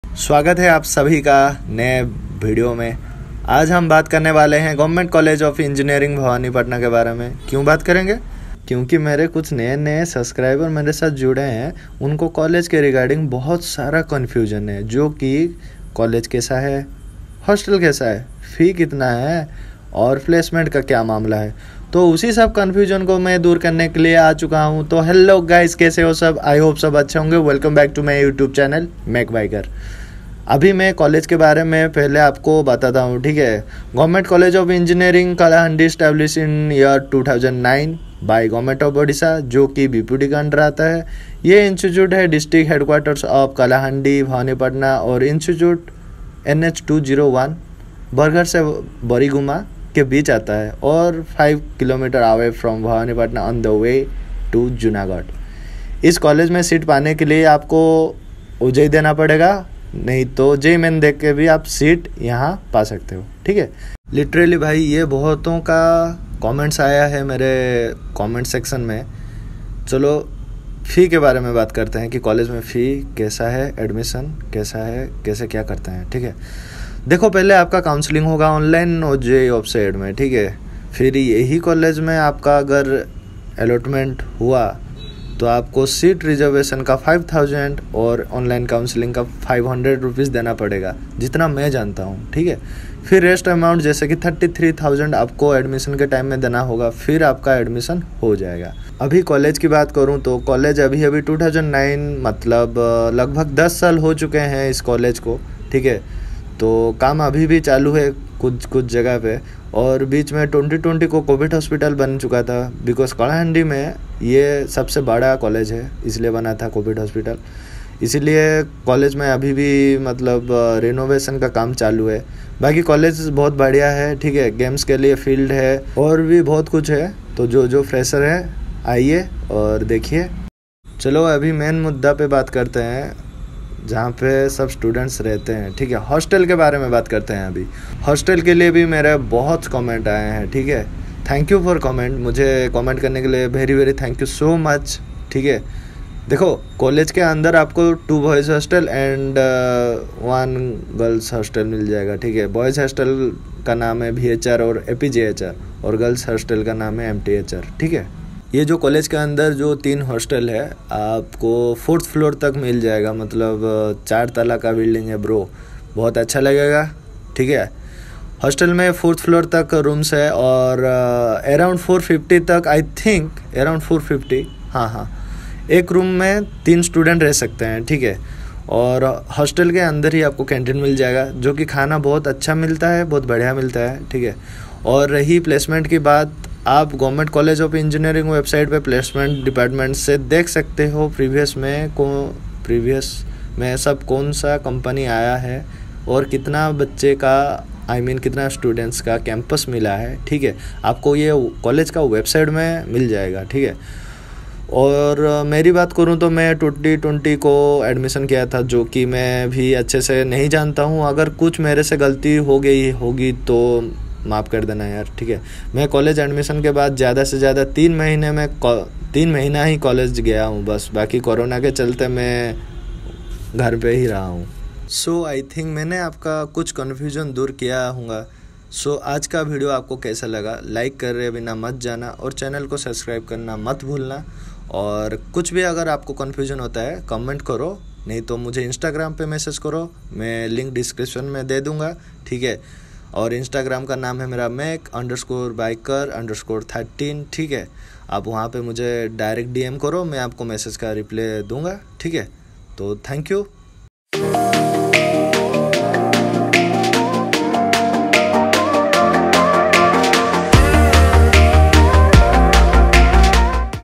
GOVERNMENT COLLEGE OF ENGINEERING, bhawanipatna🏫Admission process,Fees,Hostel,Placement,campus🥸
Ap Sabhi ka Confusion door karne mein leke aya hoon ek short video of government college of engineering, bhawanipatna, Kalahandi

Ishme apko Admission process
Admission fee
Hostel facility
College campus
and placement ke bare me ek video banaya hai bas apke liye
Video ko like karna mat bhulna